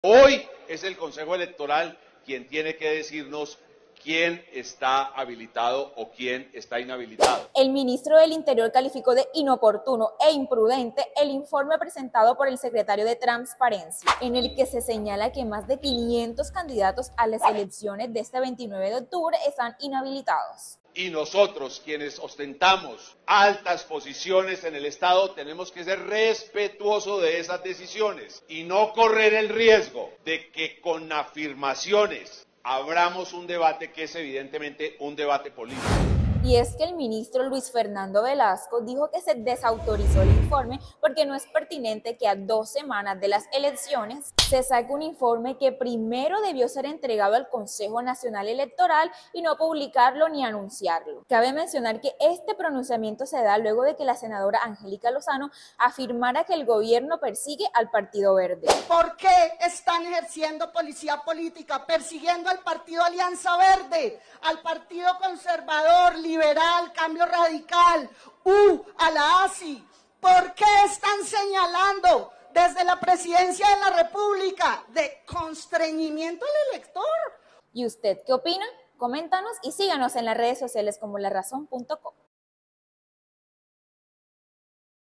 MinInterior desautoriza al secretario de Transparencia
El ministro del Interior Luis Fernando Velasco, desautorizó el informe del secretario de Transparencia, Andrés Idarraga, en el que se señala que más de 500 candidatos a las elecciones del próximo 29 de octubre están inhabilitados.

¡Siempre Informando! 📢/